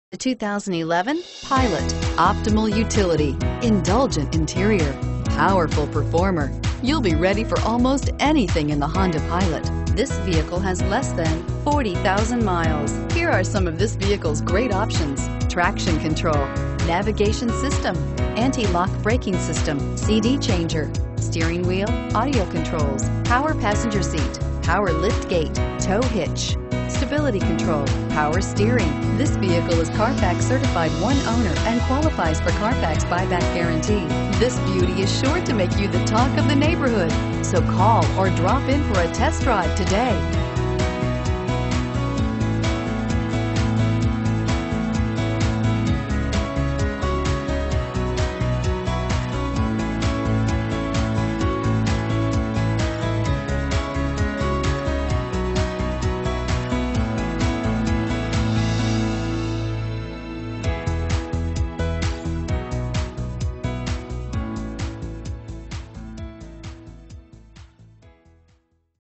2011 Honda Pilot Buford GA 8126A
This 2011 Honda Pilot 4dr 4WD 4dr Touring with RES & Navi SUV features a 3.5L SOHC MPFI 24-VALVE I 6cyl Gasoline engine. It is equipped with a 5 Speed Automatic transmission. The vehicle is WHITE with a Black Full Leather interior. It is offered As-Is, extended warranty is available. - CARFAX 1-Owner, GREAT MILES 35,779! Touring trim.